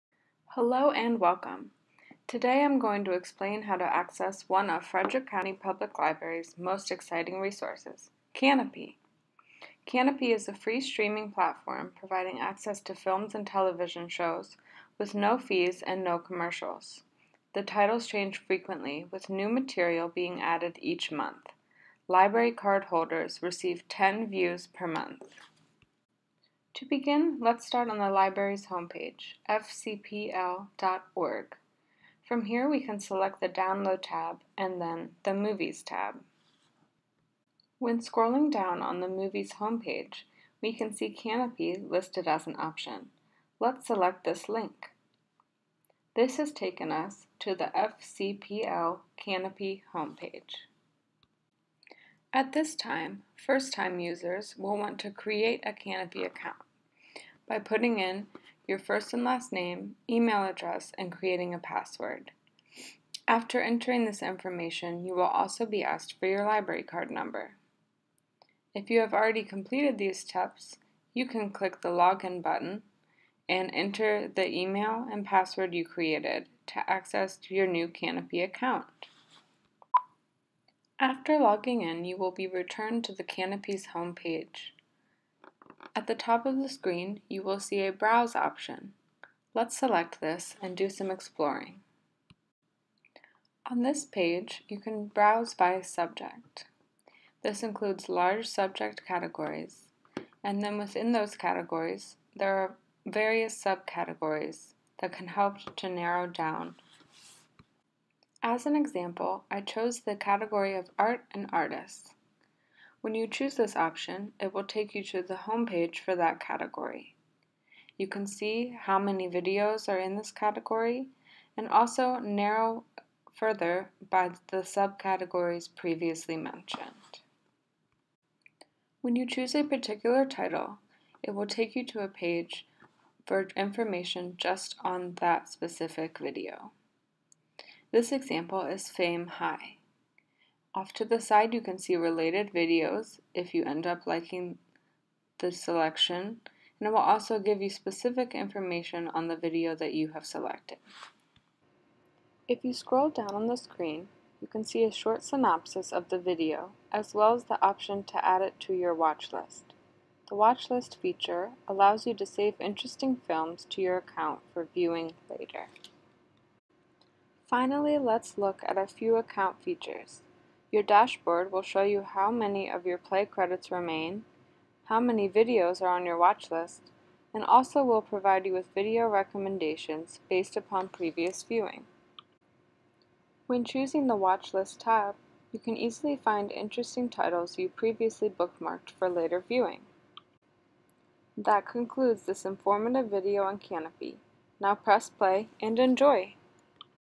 Kanopy Instruction Video
Kanopy is a film streaming service available through public libraries. With your library card, you can access independent and foreign films and television shows. This video will show new Kanopy users how to create an account and navigate the website. Topics covered are browse features, website layout, account knowledge, and watchlist capabilities.